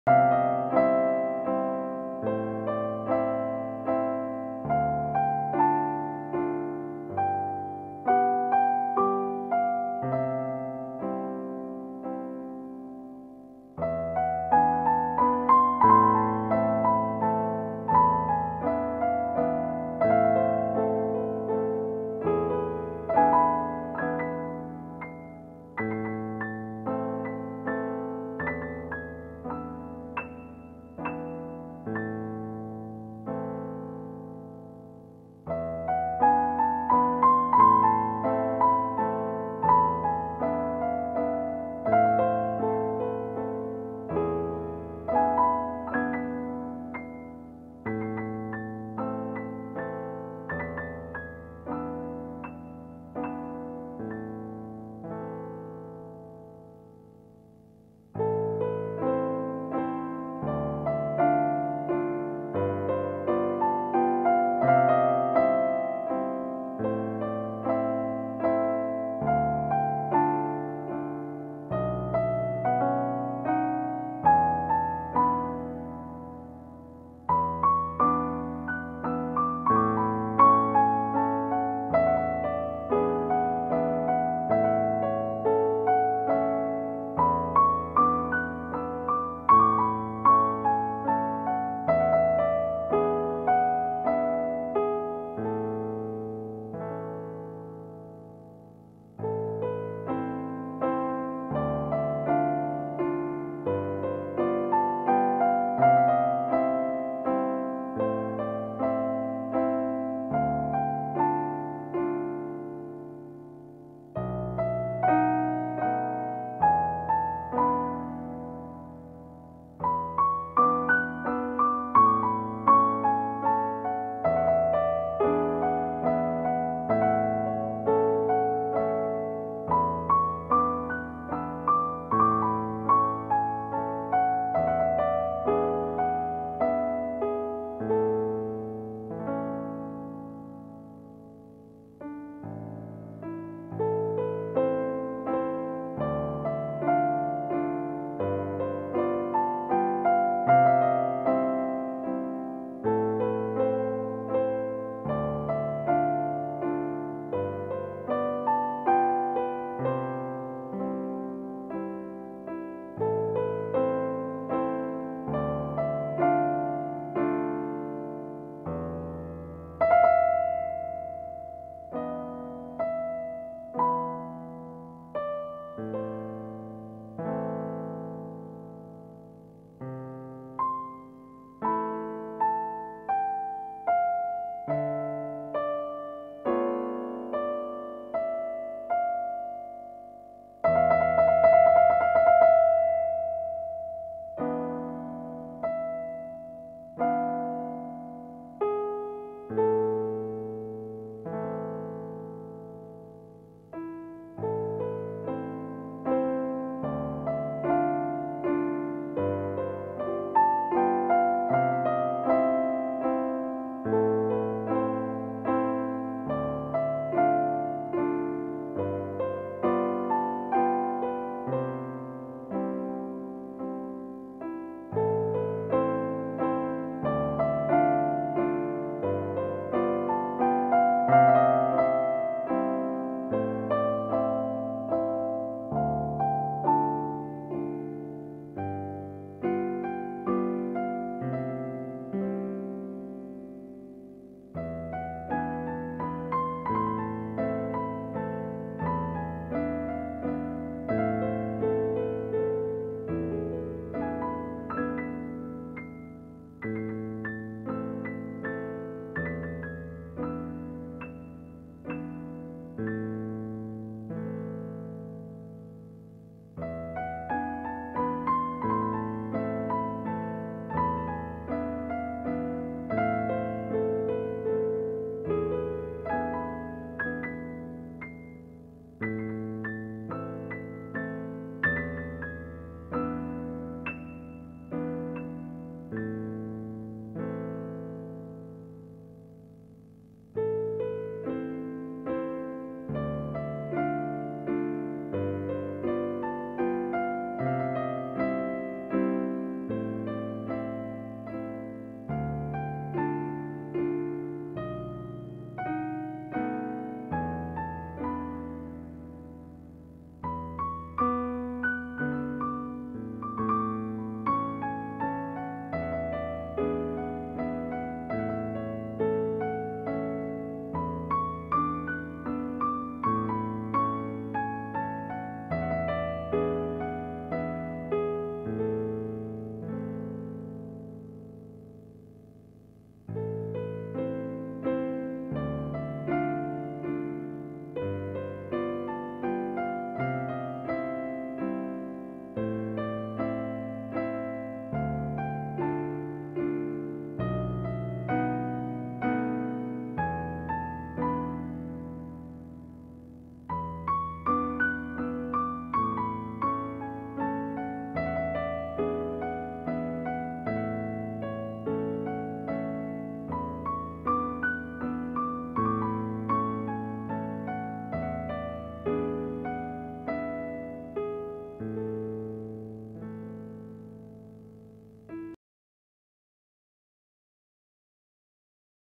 Solo Piano | Shakila | Profee Concert 25/08/19 | Revo Town
Profee Music School